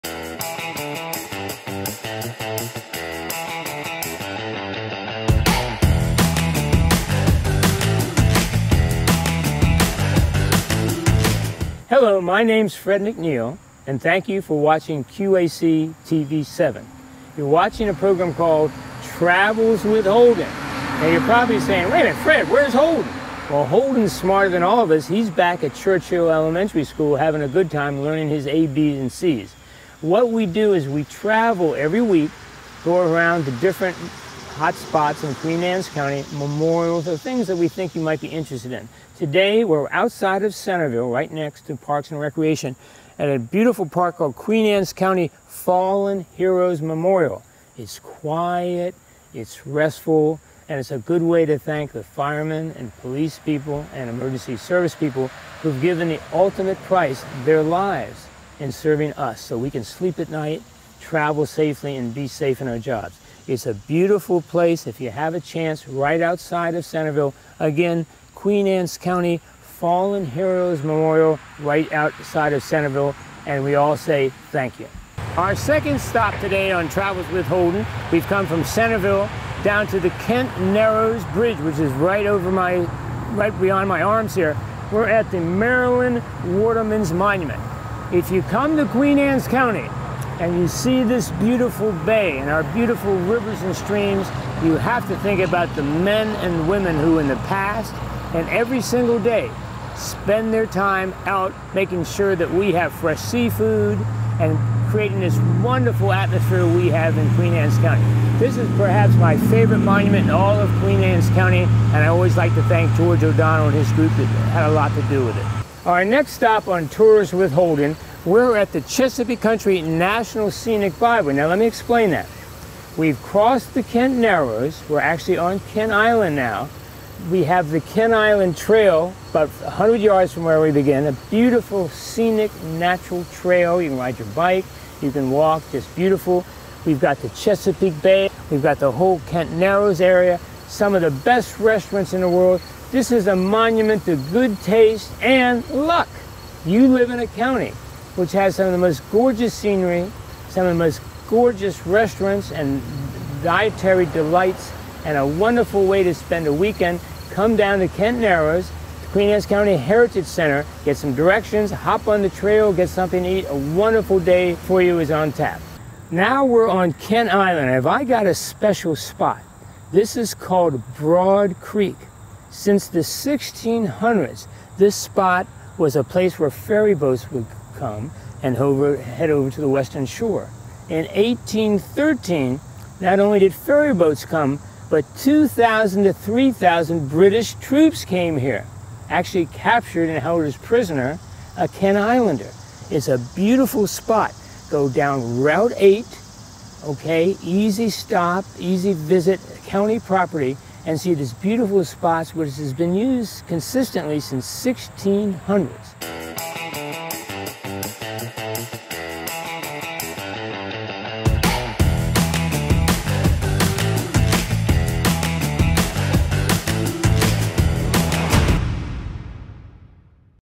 Travels with Holden - South County
Join Fred as he visits some more memorable landmarks in Queen Anne's County, MD.